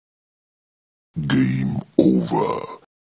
Evil game over sound effect for video games
This is a free no copyright evil game over sound effect for video games